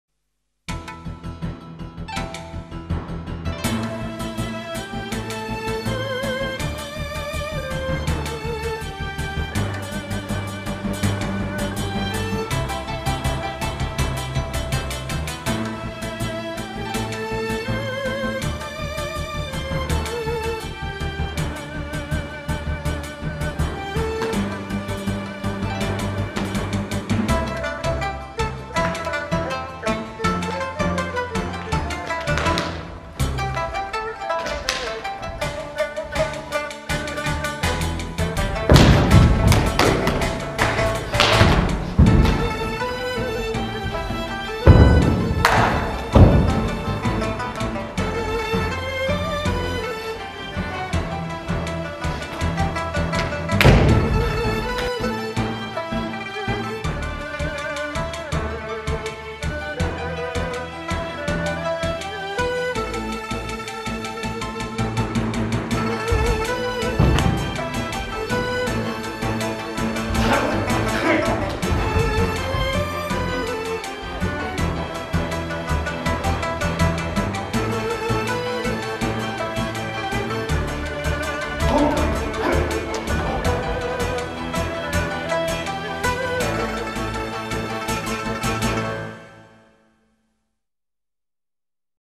Fanzi Quan DVD Trailer - Master Shao Zhao Ming
This is a trailer of the Fanzi Quan DVD, which was produced in consultation with Grandmaster Ma Xianda & Grandmaster Bai Wenxiang.  The DVD contains all the foundation training, full form of Fanzi Quan, sectional detail and close-ups.  It also contains the world's only martial application demos of each movement in the form.